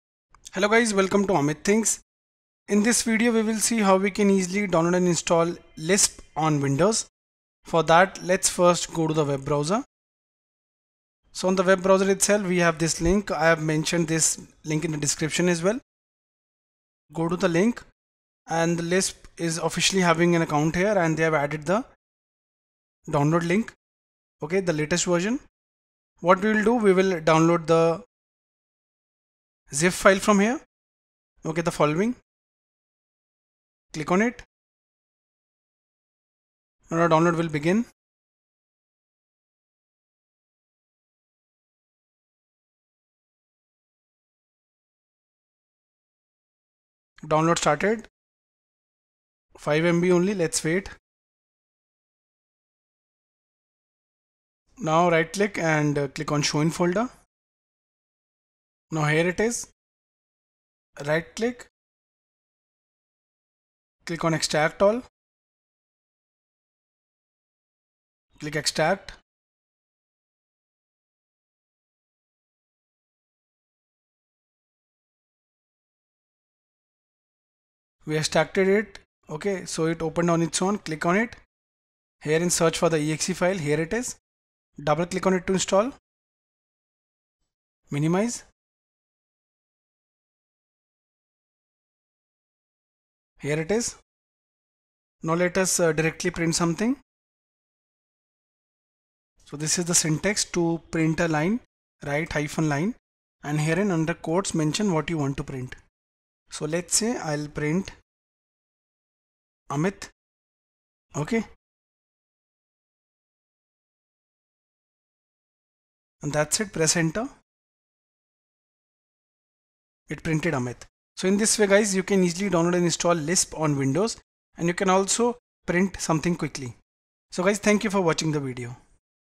How to Install LISP on Windows 11 | Complete Installation | Amit Thinks
In this video, we will learn how to download and install LISP on Windows 11.